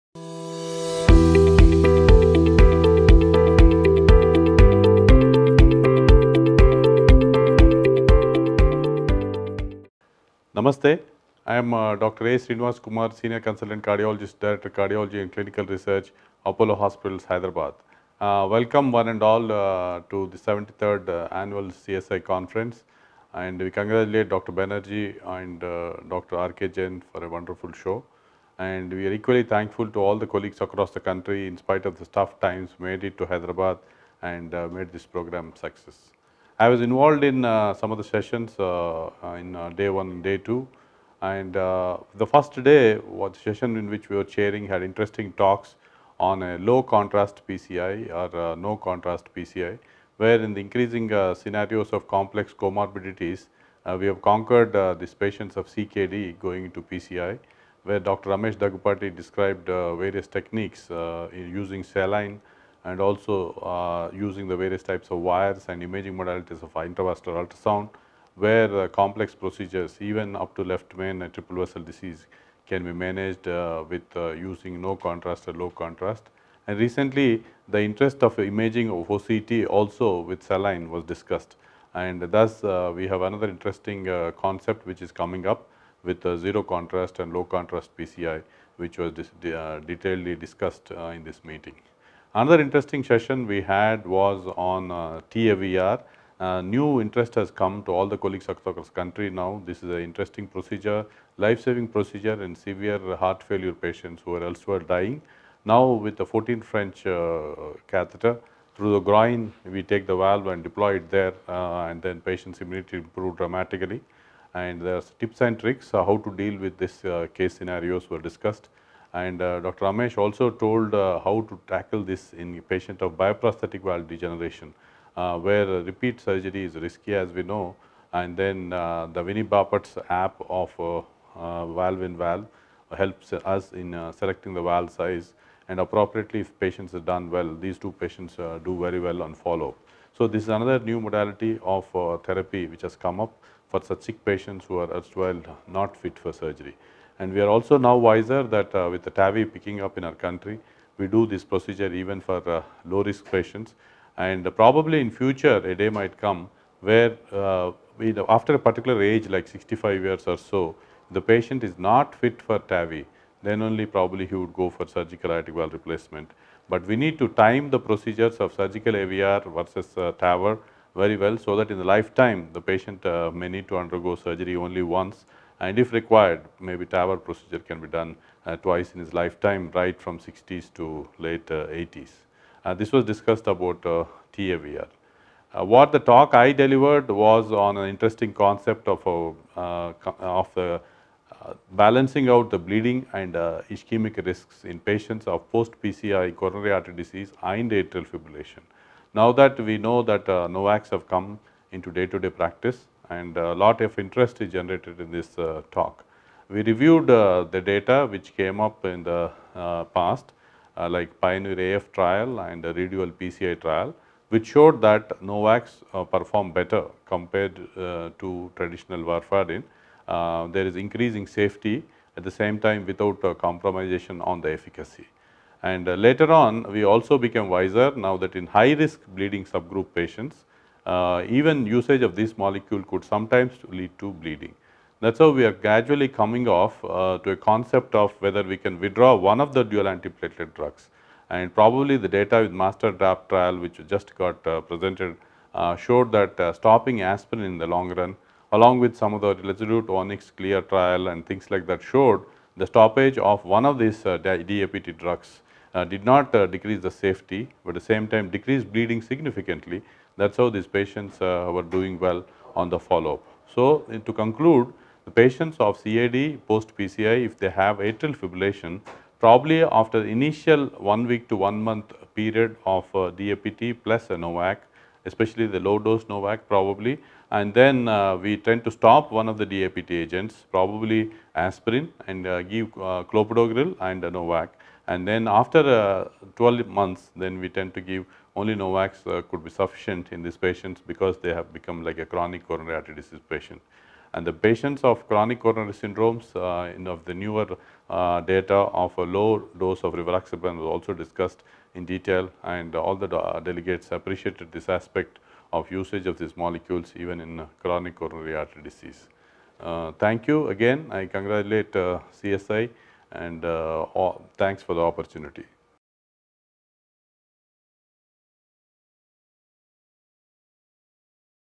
CSI 2021TV Re-imagine the session by Dr. A Sreenivas Kumar
Bringing you a knowledge empowerment initiative from this year's 73rd Annual Conference of Cardiological Society of India from the eminent faculties and delegates.

CSI 2021: Reinvent | Reimagine | Reinforce the sessions

Speaker:  Dr. A. Sreenivas Kumar
M.D., D.M., FACC
Director Cardiology & Clinical Research
Apollo Hospitals, India
Apollo Health City,
Professor of Medicine, Apollo IMSR, Jubilee Hills, Hyderabad.
Chairman, Facts Foundation & FPS